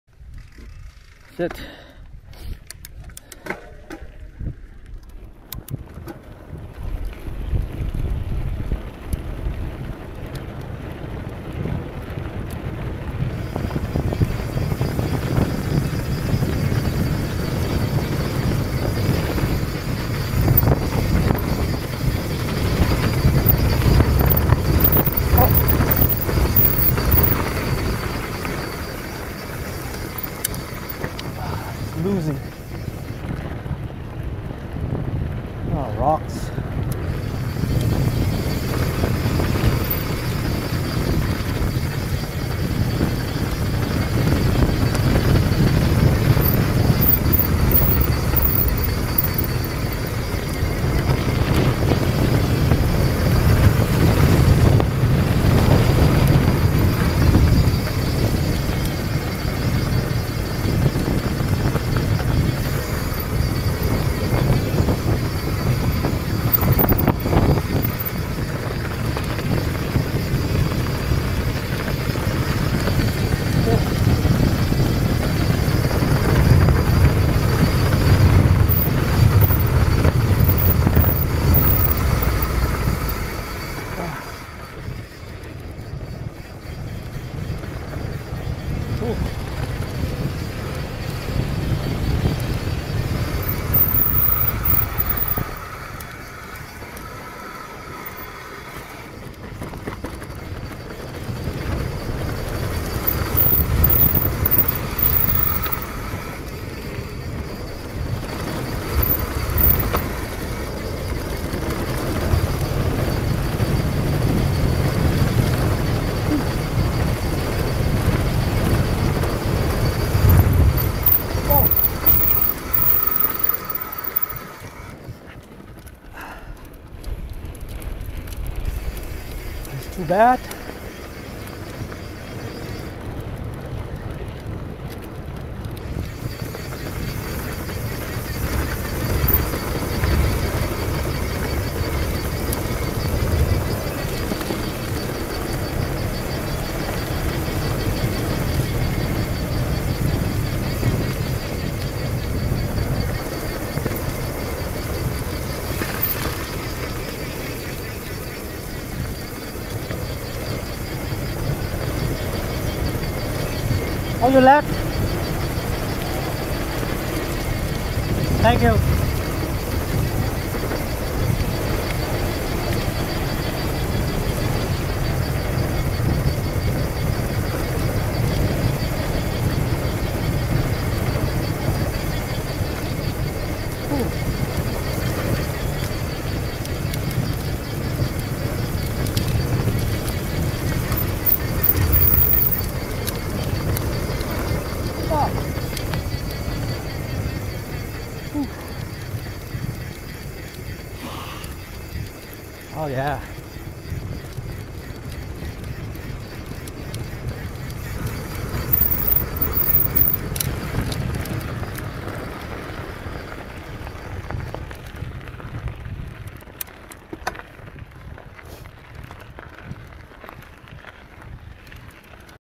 Mountain Biking with the new DJI Osmo Action 2 Camera
Here is my test video shoot with DJI Osmo Action 2 Camera at 2.7K @60fps, Super View, Rock Steady stabilization. Crop to 16:9, 1080p for Youtube.